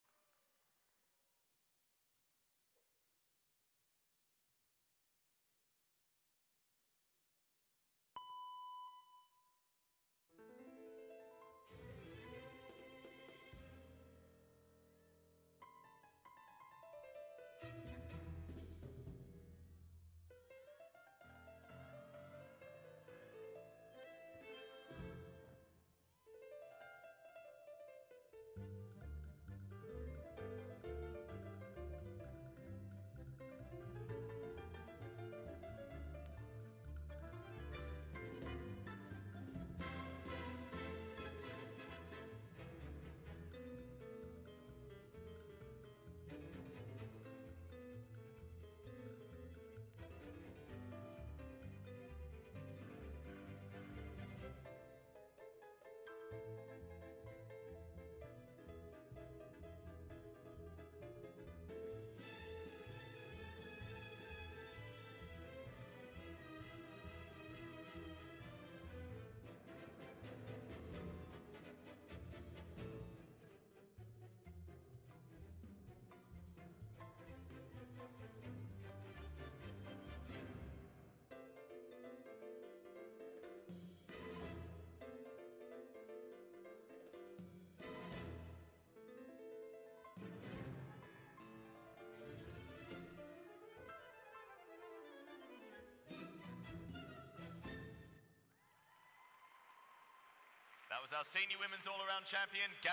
GABBY JUPP - FLOOR - 2013 British Champs - Apparatus Finals
Senior - 14.350
GABBY FLOOR routine at the 2013 Artistic Gymnastics British Championships.